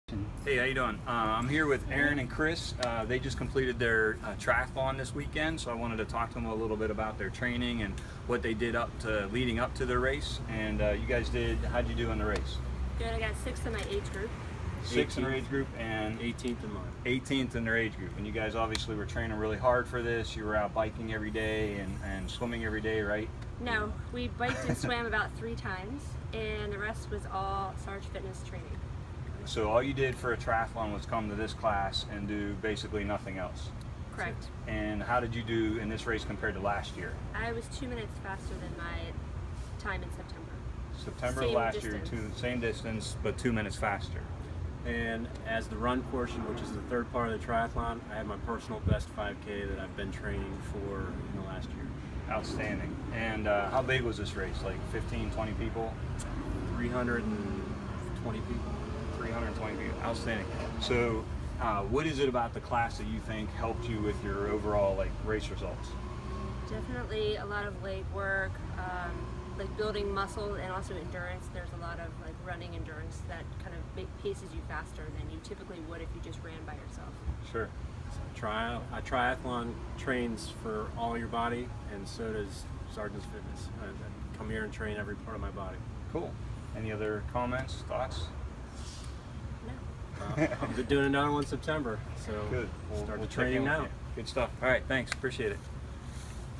Erin and Chris do better in Triathlon after bootcamp
Military styled fitness programs in Virginia, Maryland and Washington DC.  Providing Fitness Boot Camp, in home personal training and corporate services.